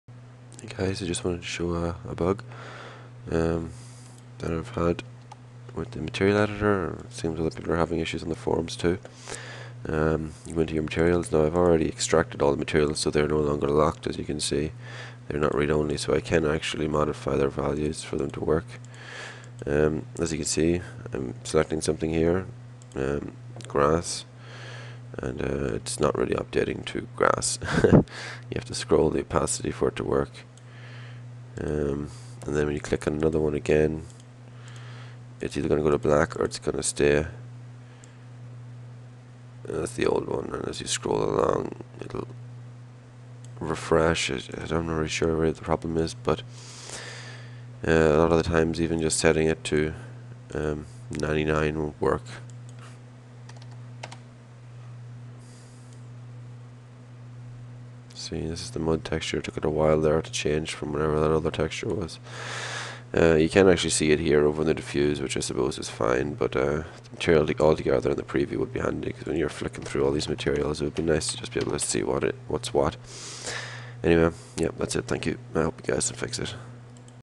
Material Editor Preview Bug CRYENGINE 3.5.3
An irritating thats been about for a while now, Hoped it was fixed with this version, Maybe in the future :)